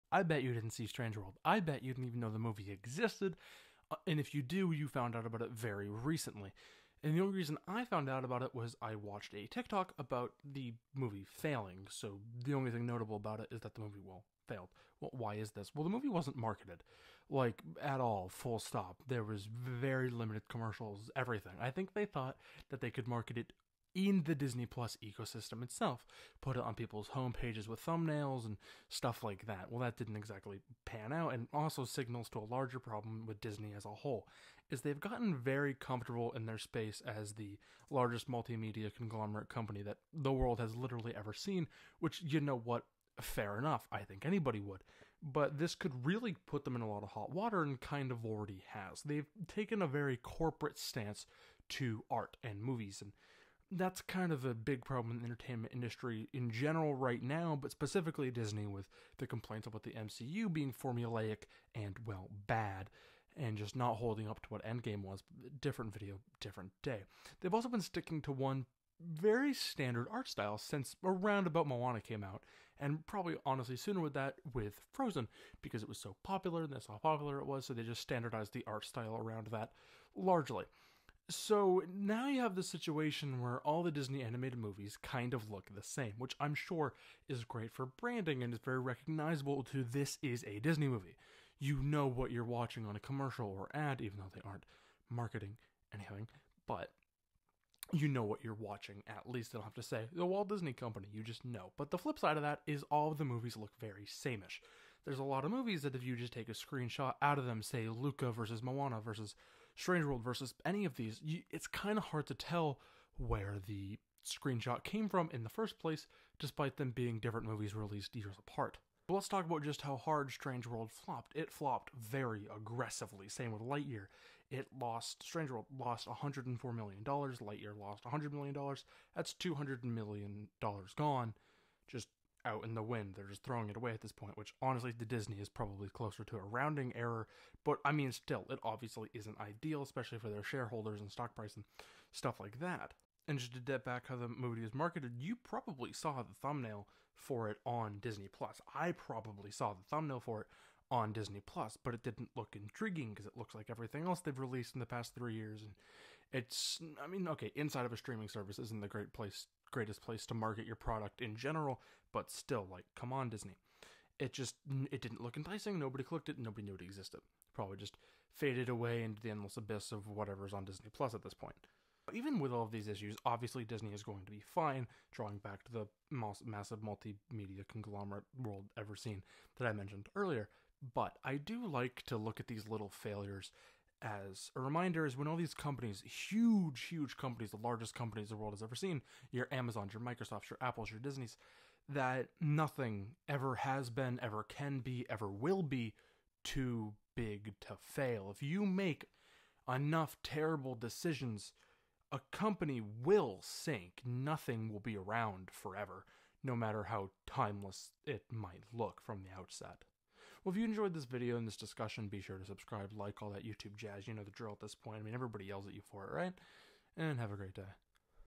Why Strange World FLOPPED
Disney's latest failure, I try to shed some light into the multiple reason Strange World failed as well as some of Disney's other recent animated movies.